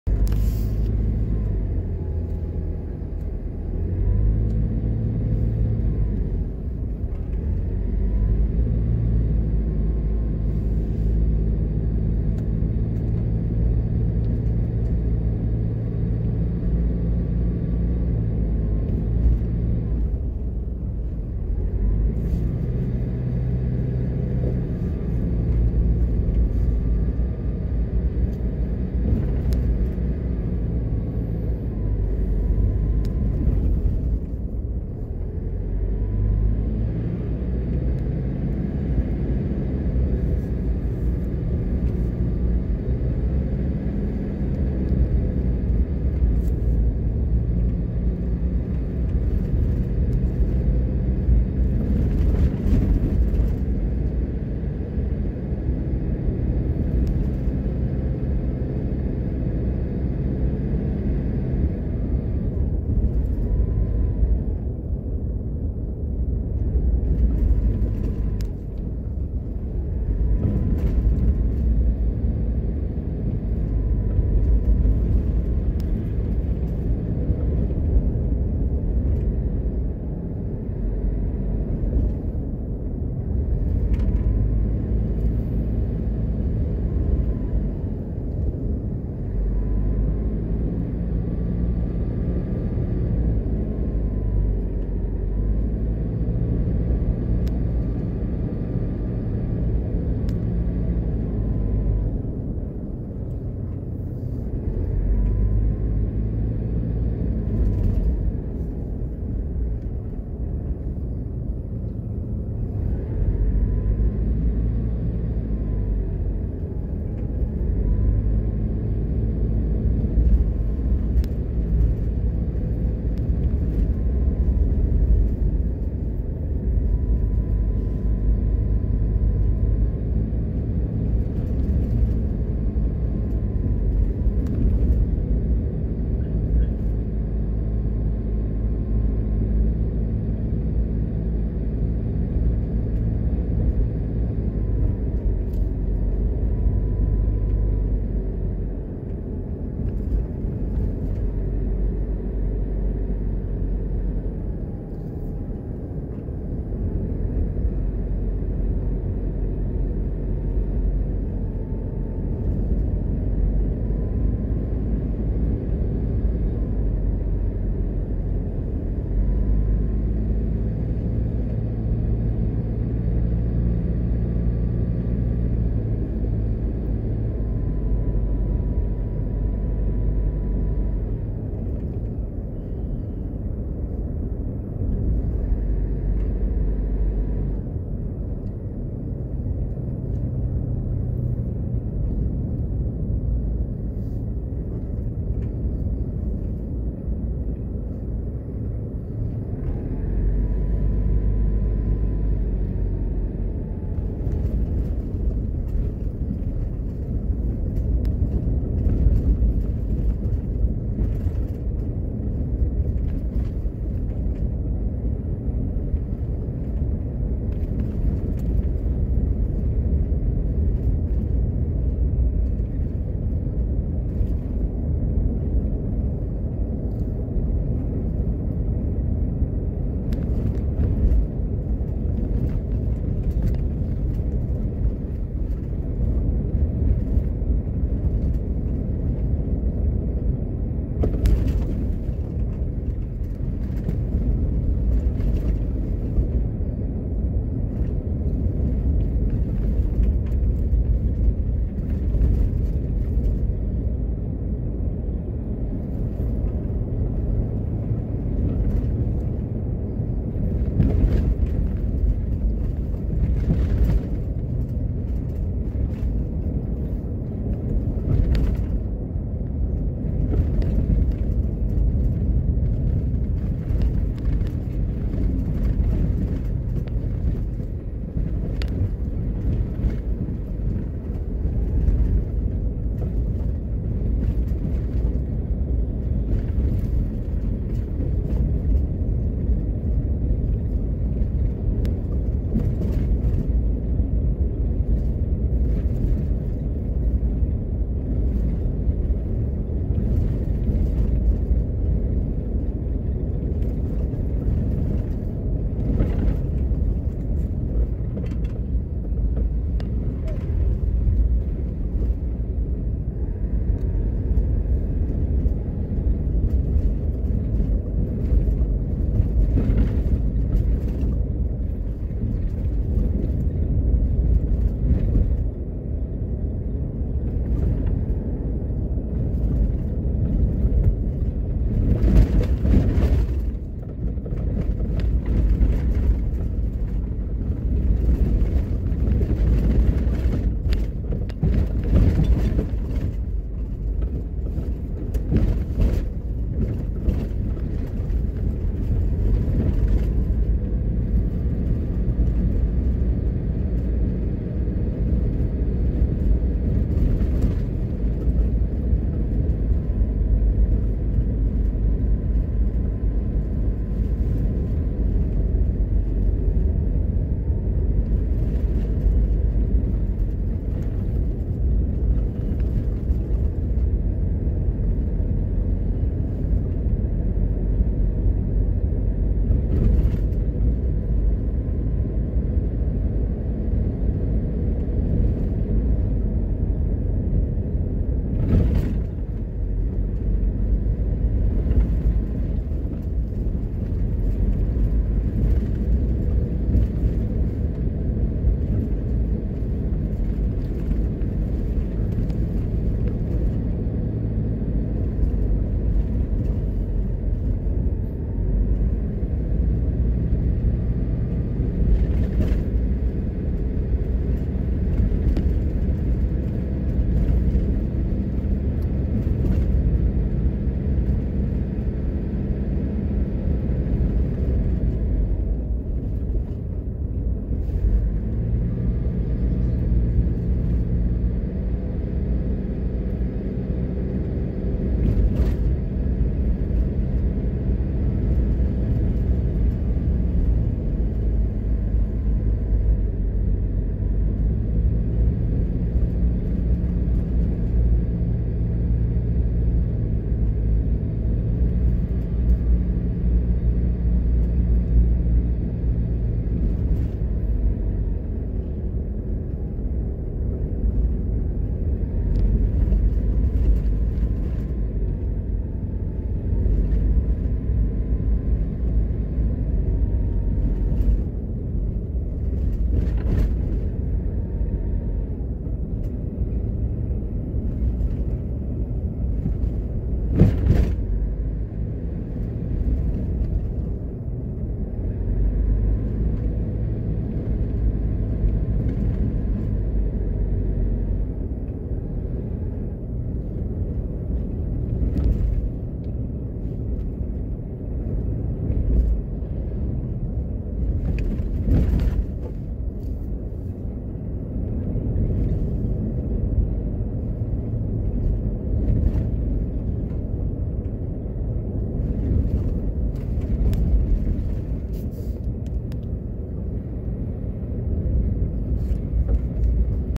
Driving through Fern Gully at night | Jamaica
Driving through Fern Gully at night in St Ann Jamaica